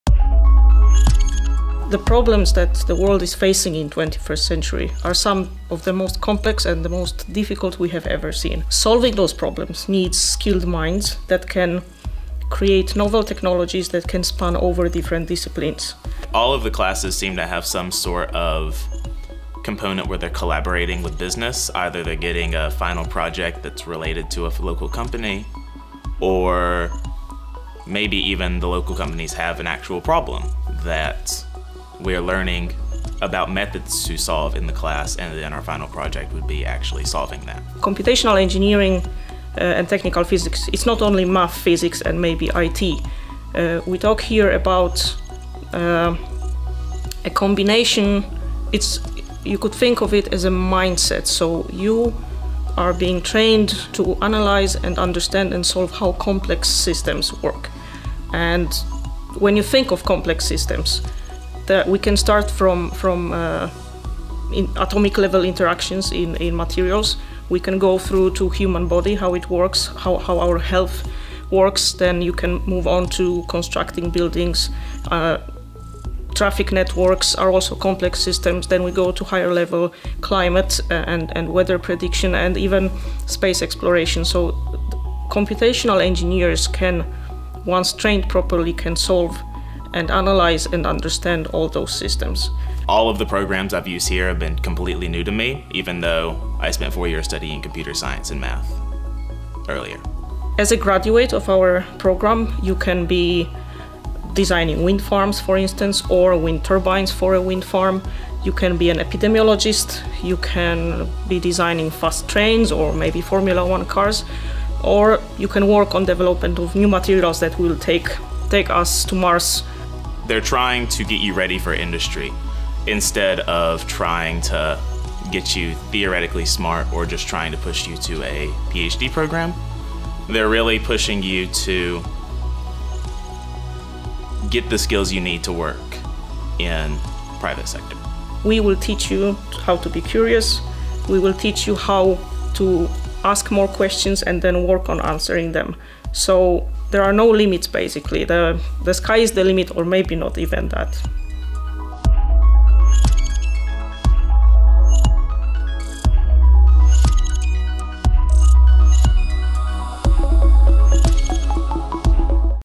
🎓 Computational Engineering and Technical Physics - Master's Programme - LUT University
One cannot overstate the importance of scientific knowledge and computational engineering in the global information society and  industrial economy, particularly as the systems we use grow more and more complex.